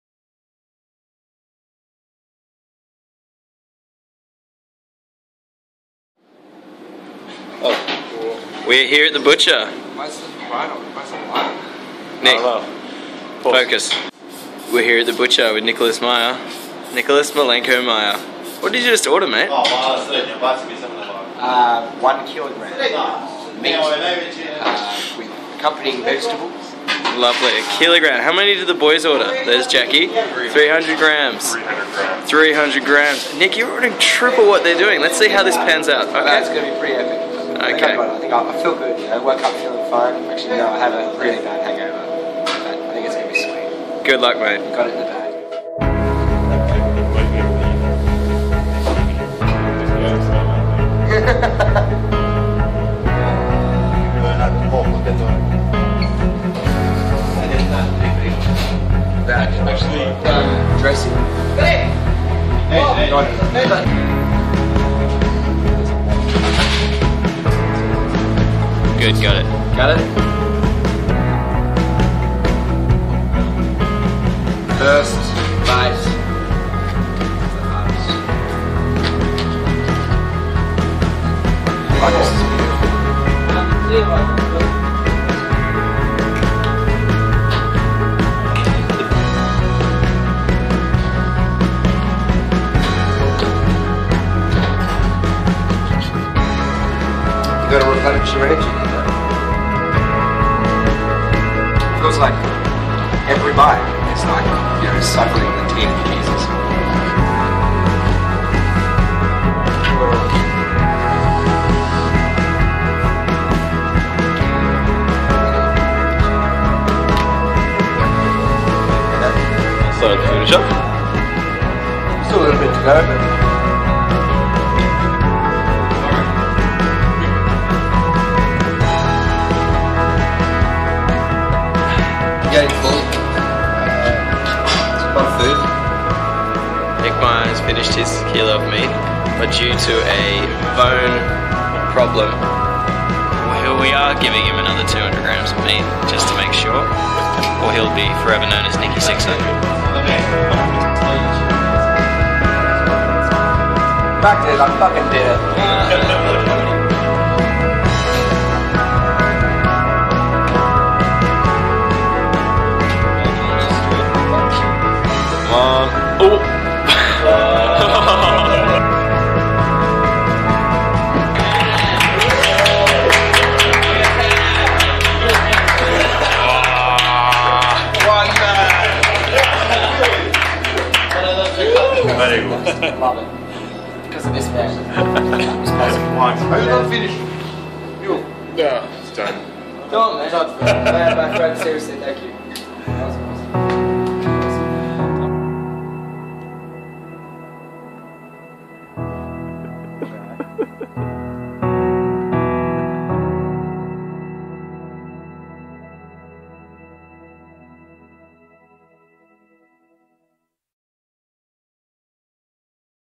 Balkan Boyz: BBQ Tanjga 2013
Finally put this old SD footage to use! Take me back to Nic's epic BBQ meat feast in Kotor, Montenegro :)